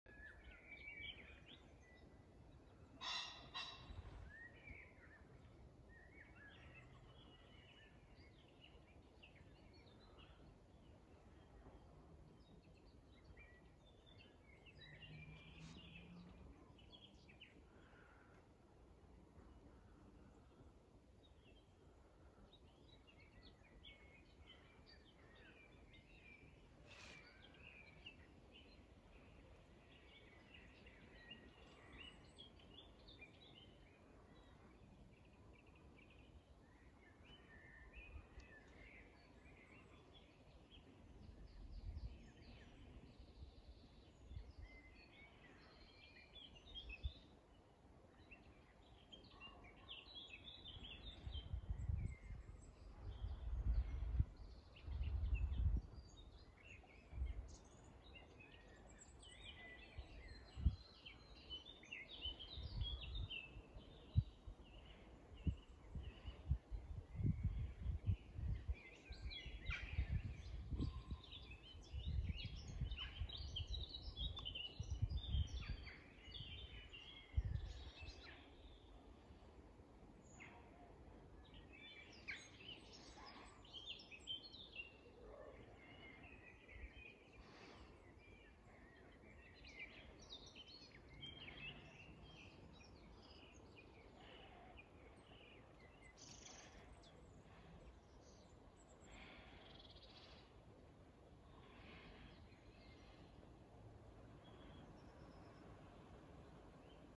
A pheasant cries out in this incredibly beautiful corner of England
Enjoy this amazing place, such beauty yet such a quiet spot, a pheasant cries out with the sun shining through the trees in a blue sky, the birds sing and the breeze gently rustles the new spring leaves and a girls stands by the style.

Let your fears subside, your anxieties calm and any depression lift by watching and listening and being here and now in this moment.  I hope this helps to make your world a better place.